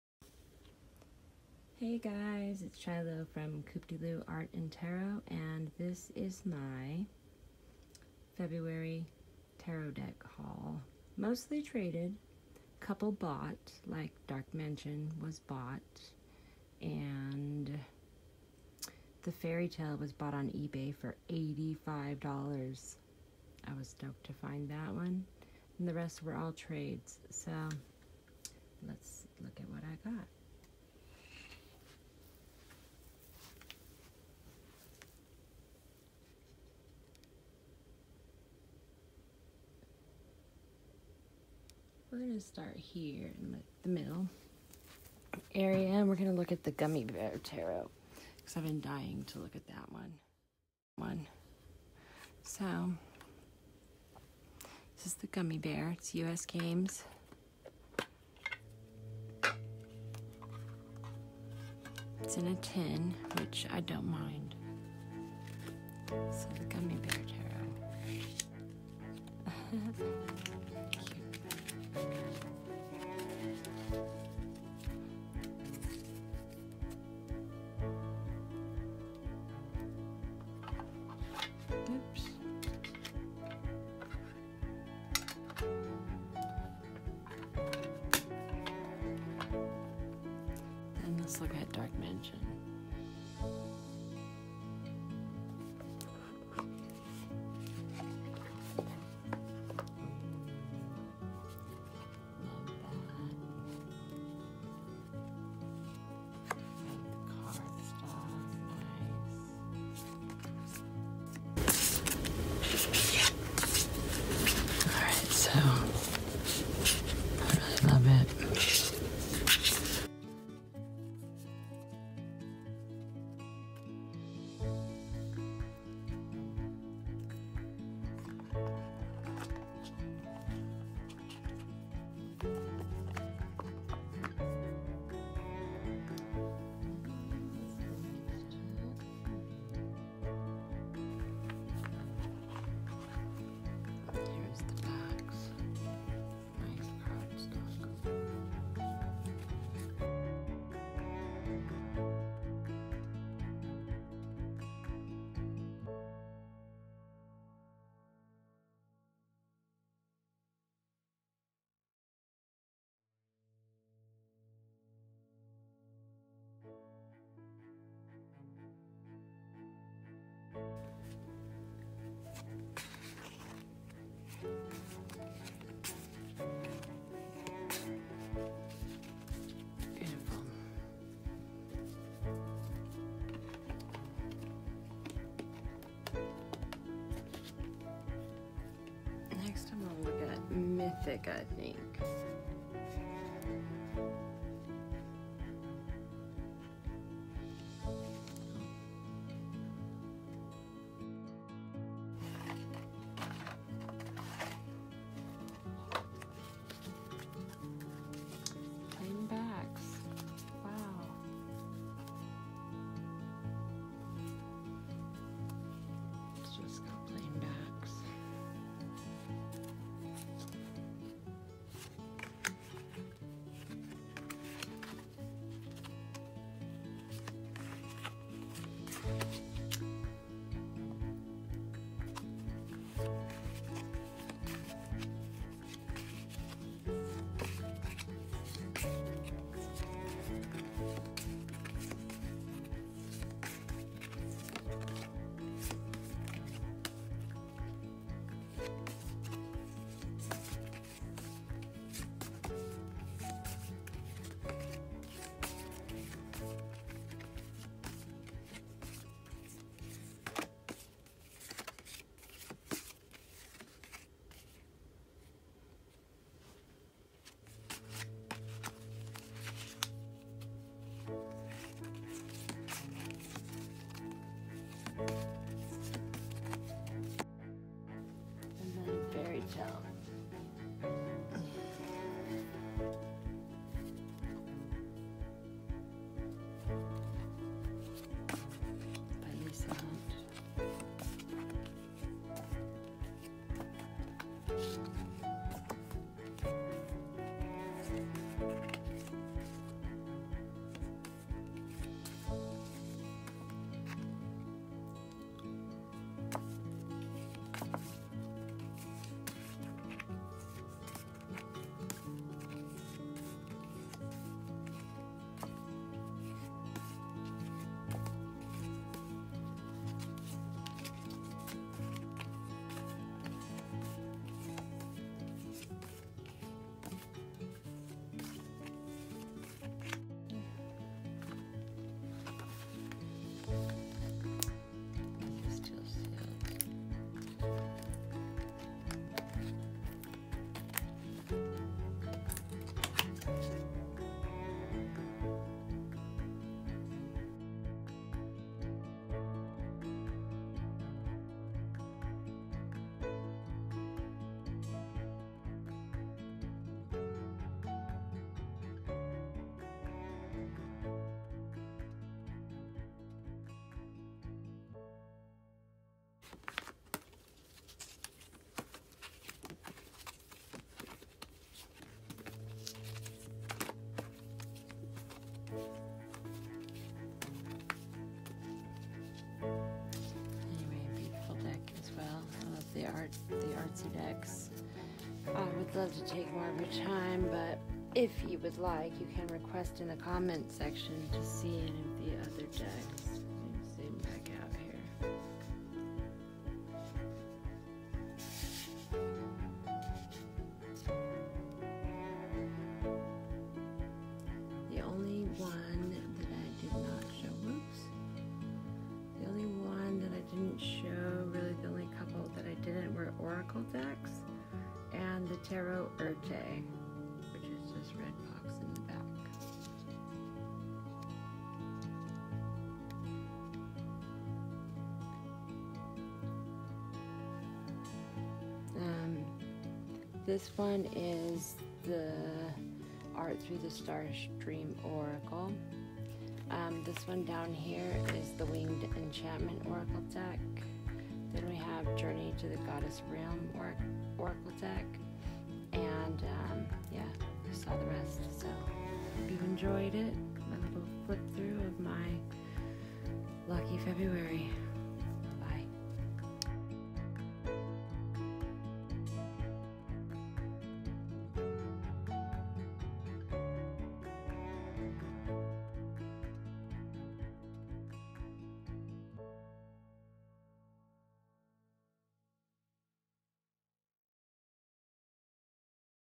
cooptylew My February Tarot Haul
Tarot decks that I acquired in the month of February 2019, mostly through trades.